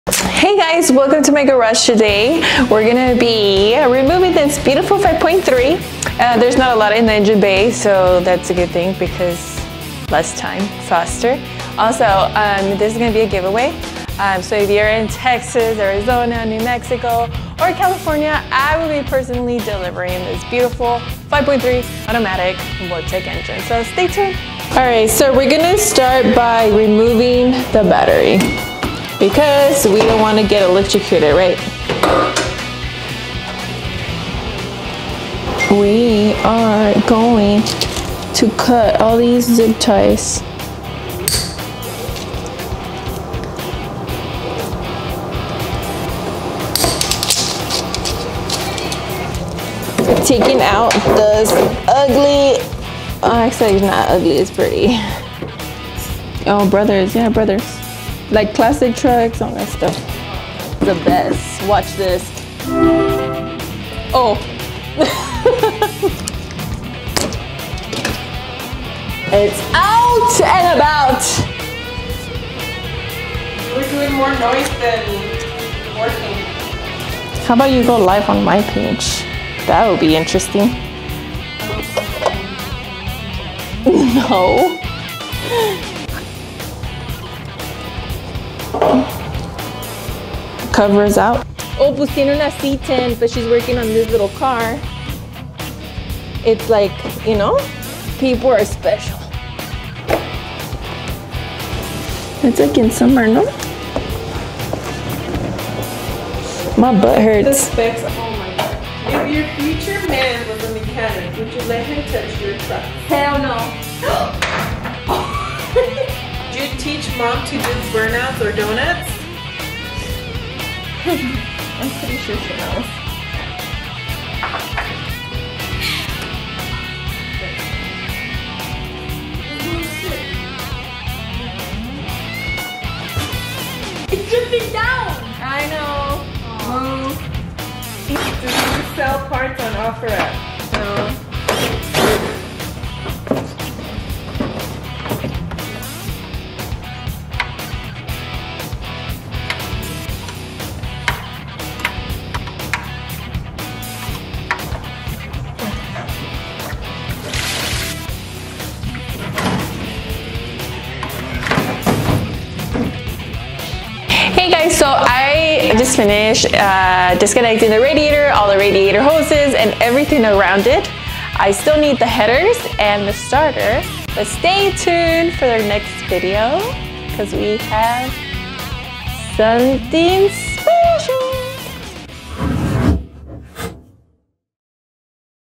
The Beginning of a BADASS C10 Build by RSTGirl
This is the first part of the engine removal process of my C10, which is a 5.3 and preparing to install his new heart a beautiful LSA from a 2013 ZL1 Camaro!